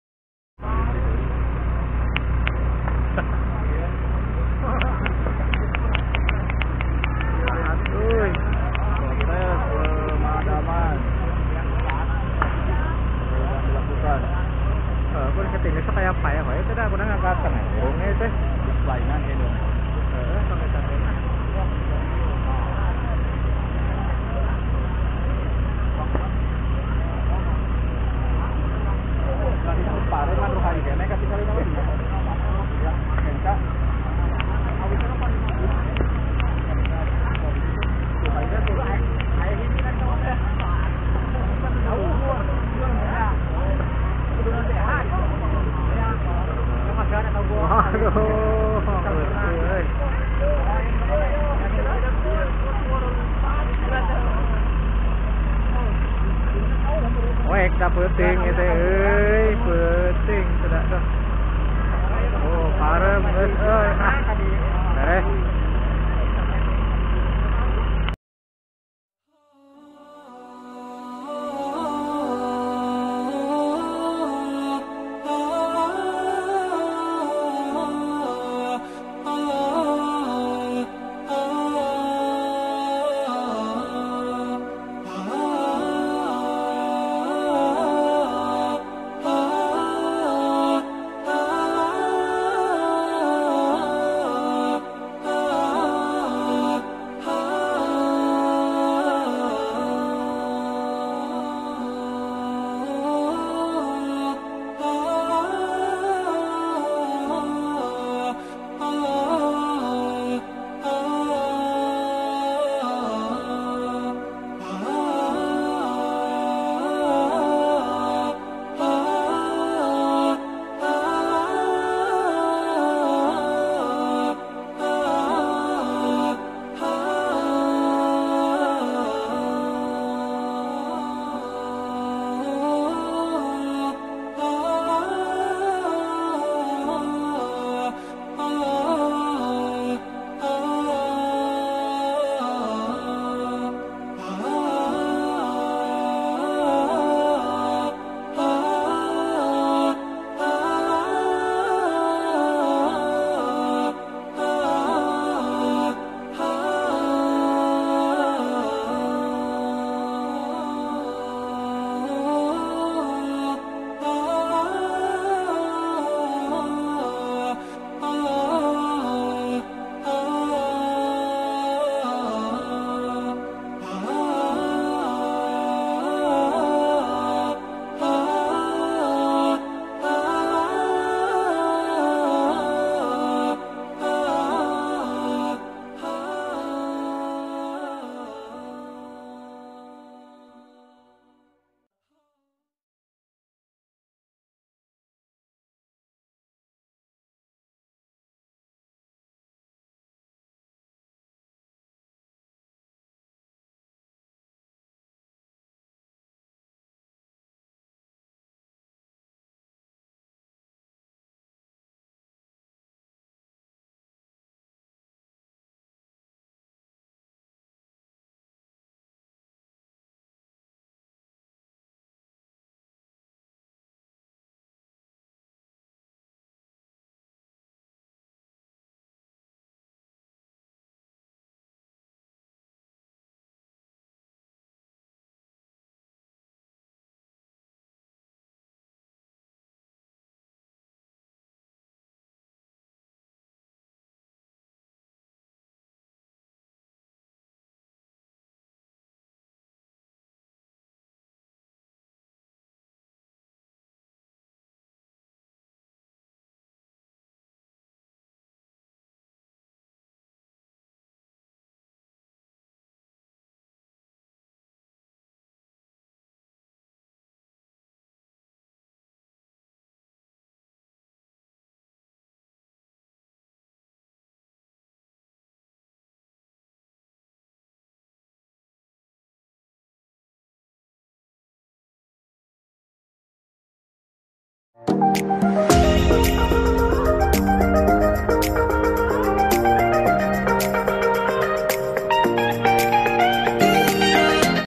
kebakaran hampir membakar rumah warga
kebakaran melanda kampung putat irgasi desa sirnamanah kecamatan darangdan kabupaten Purwakarta istimewa
hatur nuhun ulah hilap